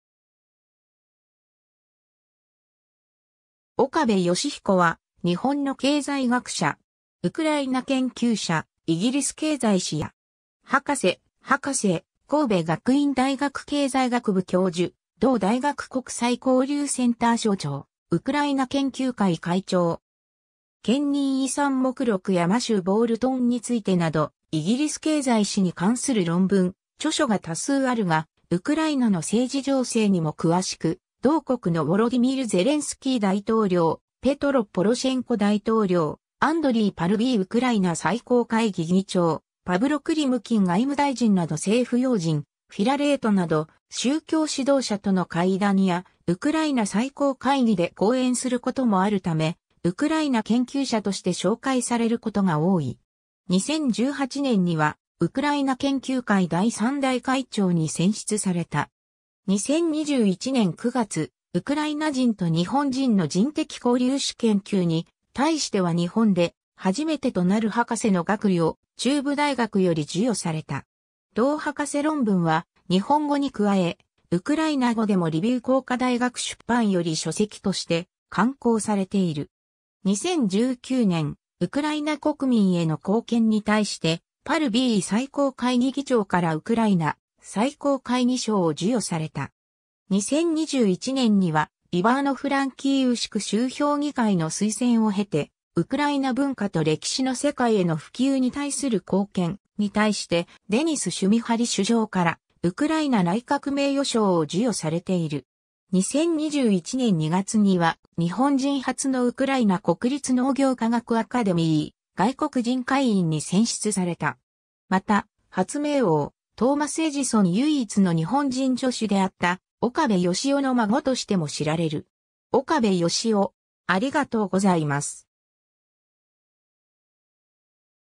岡部芳彦
日本・ウクライナ関係
岡部 芳彦（おかべ よしひこ、1973年 - ）は、日本の経済学者。
ウクライナ研究者、イギリス経済史家。
博士（歴史学）、博士 (経済学)、神戸学院大学経済学部教授、同大学国際交流センター所長、ウクライナ研究会(国際ウクライナ学会日本支部)会長。
 検認遺産目録やマシュー・ボールトンについてなどイギリス経済史に関する論文・著書が多数あるが、ウクライナの政治情勢にも詳しく、同国のウォロディミル・ゼレンスキー大統領、ペトロ・ポロシェンコ大統領、アンドリー・パルビー・ウクライナ最高会議（国会）議長、パブロ・クリムキン外務大臣など政府要人、フィラレート (キエフ総主教)など宗教指導者との会談や、ウクライナ最高会議で講演することもあるため、ウクライナ研究者として紹介されることが多い。
2018年には、ウクライナ研究会第三代会長に選出された。
2021年9月、ウクライナ人と日本人の人的交流史研究に対しては日本で初めてとなる博士（歴史学）の学位を中部大学より授与された。
同博士論文は、日本語にくわえ、ウクライナ語でもリヴィウ工科大学出版より書籍として刊行されている。
 2019年、「ウクライナ国民への貢献」に対して、パルビー最高会議議長からウクライナ最高会議章を授与された。
2021年には、イヴァーノ＝フランキーウシク州評議会の推薦を経て、「ウクライナ文化と歴史の世界への普及に対する貢献」に対して、デニス・シュミハリ首相からウクライナ内閣名誉章を授与されている。
2021年2月には、日本人初のウクライナ国立農業科学アカデミー外国人会員に選出された。
 また、発明王トーマス・エジソン唯一の日本人助手であった岡部芳郎の孫としても知られる。
 岡部芳郎（祖父。トーマス・エジソン唯一の日本人助手）